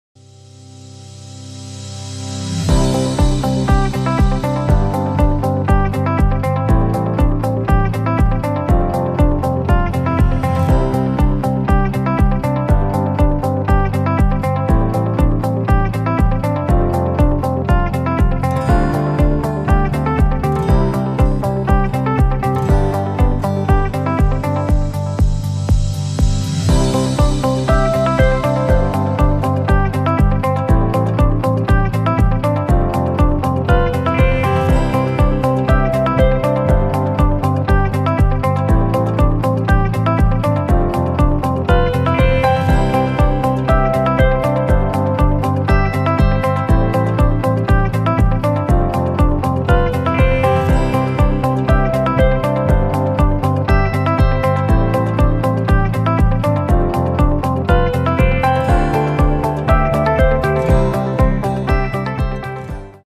Toilet Tissue Roll Bundling Packing Machine
It adopts advanced servodrive,touch screen and PLC. Parameter is setted conveniently and quickly. Machine automatically completes product from the automatic feeding , arranging , opening the bag, filling into the bag, inserting angle and sealing.